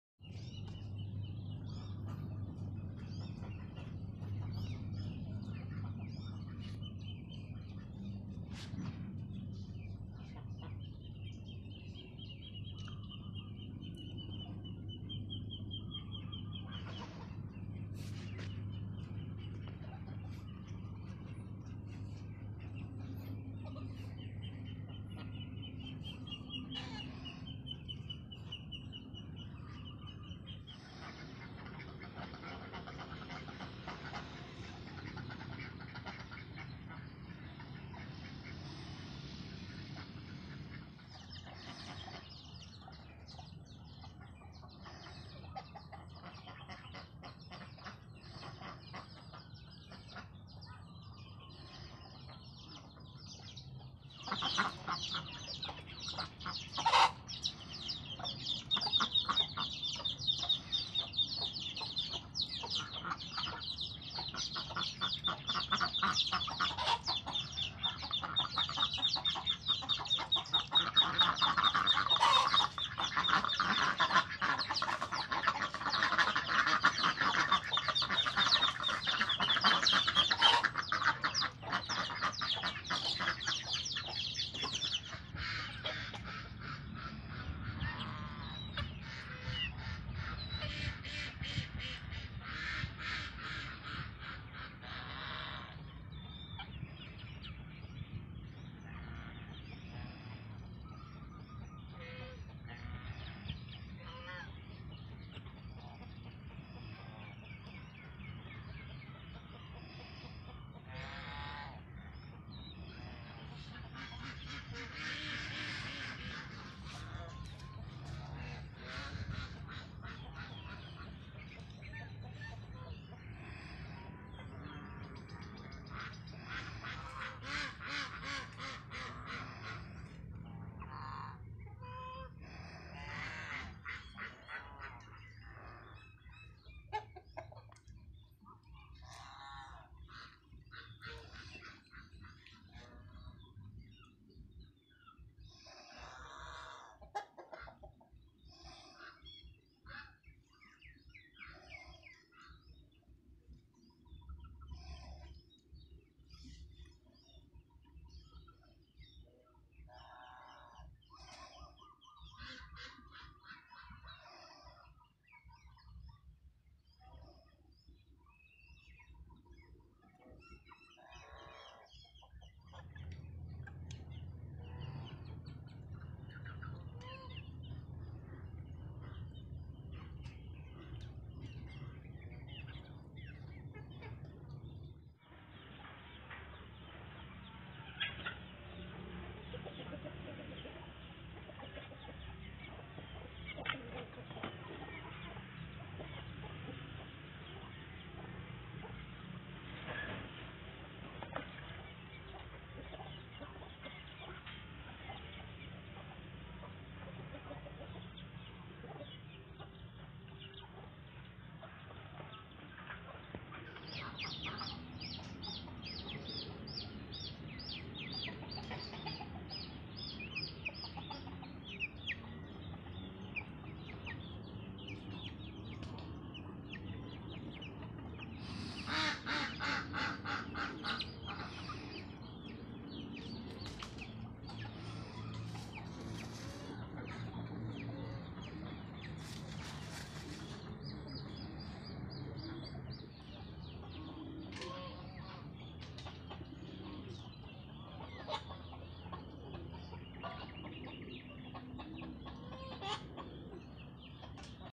How to raise your chickens in the countryside in the Cambodia.
Hello everyone to my channel kon farmer show about : How to raise your chickens in the countryside in the Cambodia.